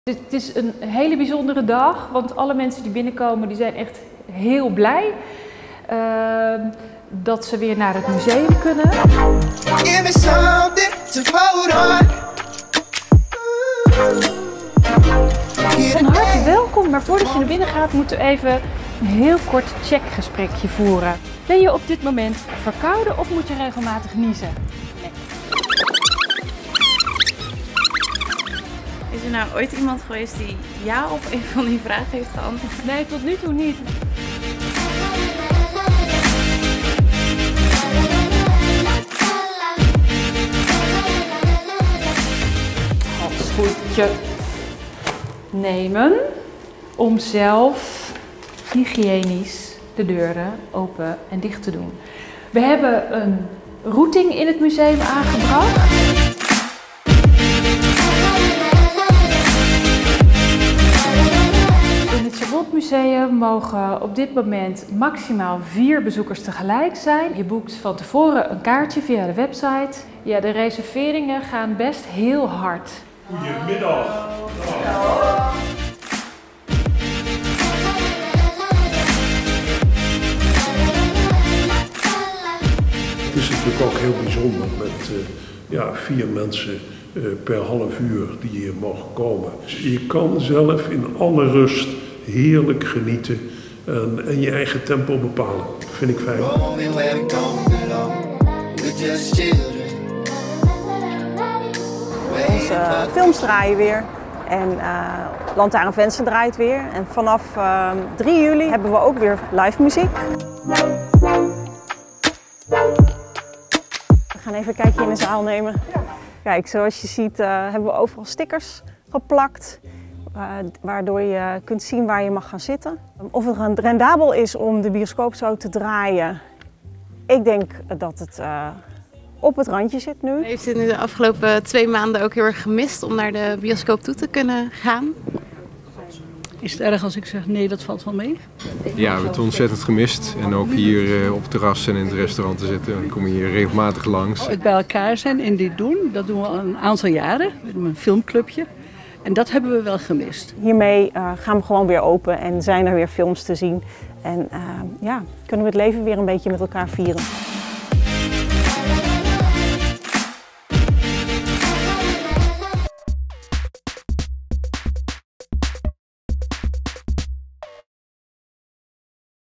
Chabot museum en LantarenVenster openen weer de deuren | Kunst en Cultuur
Het Chabot Museum en Lantarenvenster hebben de deuren weer geopend. Beide locaties hebben de nodige maatregelen moeten nemen om mensen weer binnen te kunnen laten. Voor beide locaties is het een groot feest om weer bezoekers te hebben.